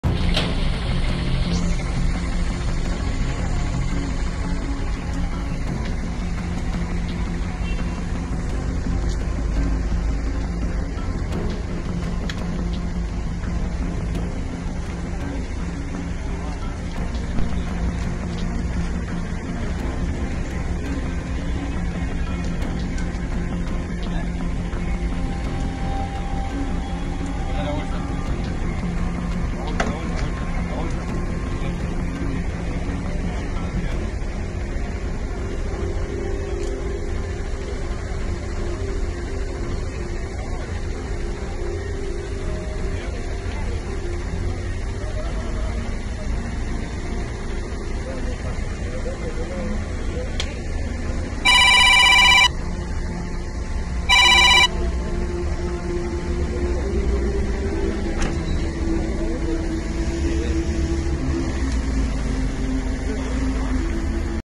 Sujeto en silla de ruedas fue acusado de abusar sexualmente de su hija
La víctima es menor de edad, y el depravado se encontraba escondido en una vivienda de la Villa 21-24. El hecho habría ocurrido en el barrio Las Praderas. La Policía Federal logró su detención.